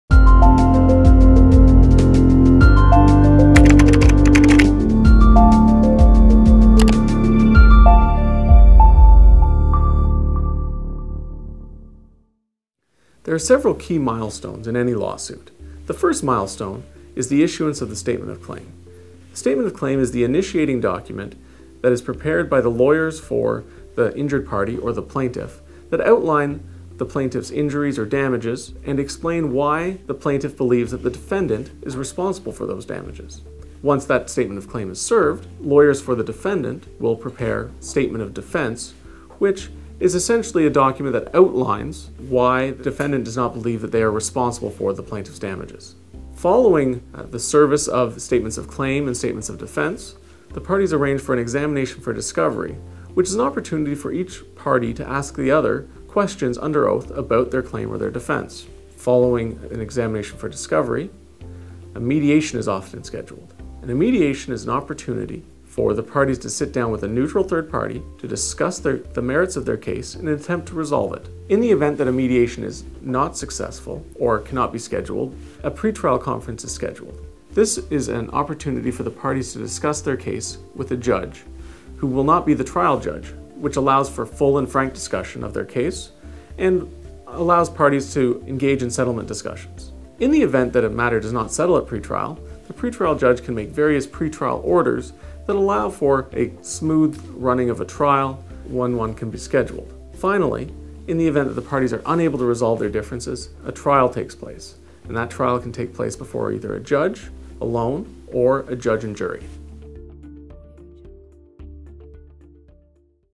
"What are the milestones in a lawsuit?"
-- Oatley Vigmond Frequently Asked Questions Series --

answered by Personal Injury Lawyer Kevin Henderson, a Partner at Oatley Vigmond LLP.